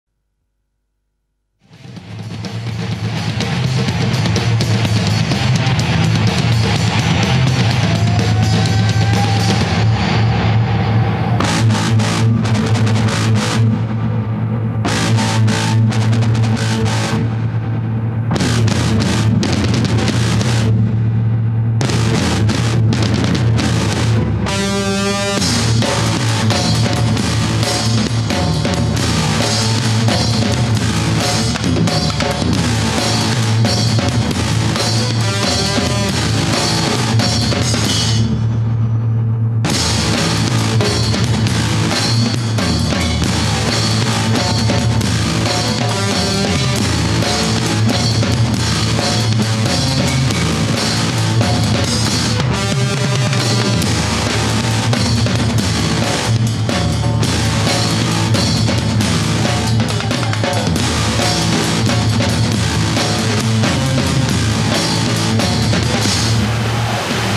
Drum Ecstasy - Аrchive - 90е
90е годы, группа Drum Ecstasy в одном из своих первых, европейских туров